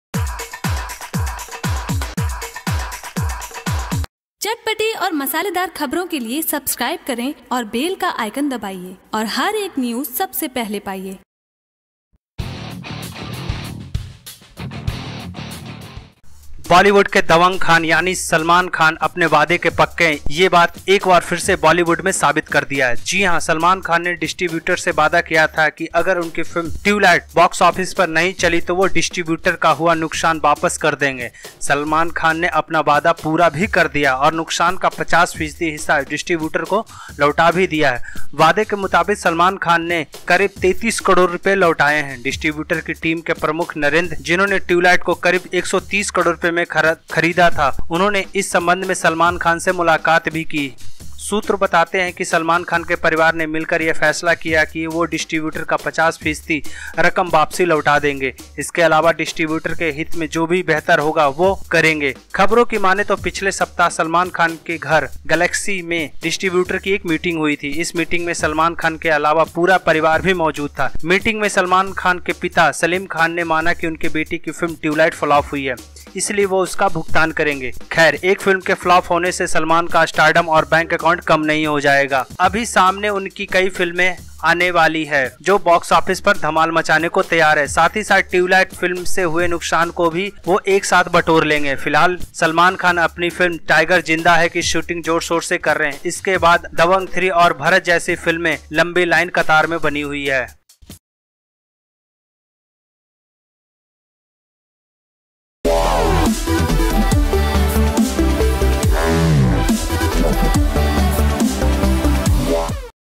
सलमान खान ने दिखाया दरियादिली, डिस्ट्रीब्यूटर्स को लौटाए उनके पैसे
सलमान खान ने दिखाया दरियादिली, डिस्ट्रीब्यूटर्स को लौटाए उनके पैसे II IndiaYo